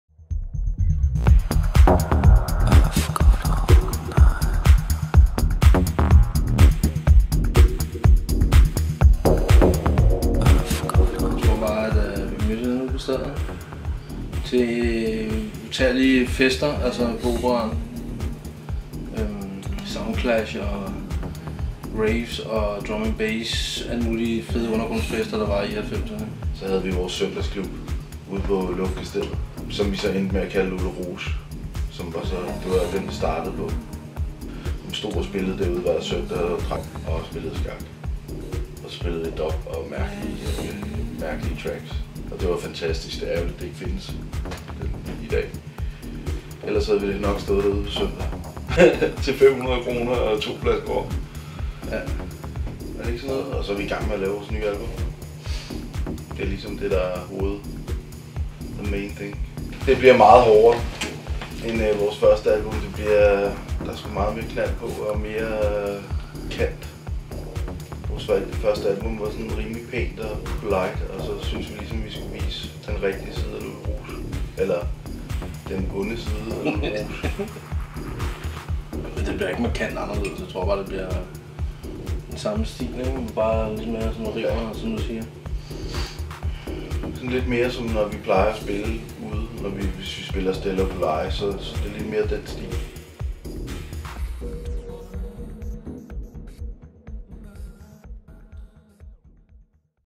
Lulu Rouge
Vi mødte Buda og T.O.M. i deres studie i en baggård på Frederiksberg, hvor de tager et hjørne af Christiania med sig, når de som to musikhåndværkere starter dagen kl. 9 med morgenbrød og en smøg. Hør dem fortælle om fortiden, dengang de hang ud på Christiania med Trentemøller på slæb, mens de løfter sløret for fremtiden og deres kommende album. Lulu Rouge spiller den 21. februar 2009 på Kødboderne 18.

Interview: Jakob Bagterp
Foto og klip: Mads Schmidt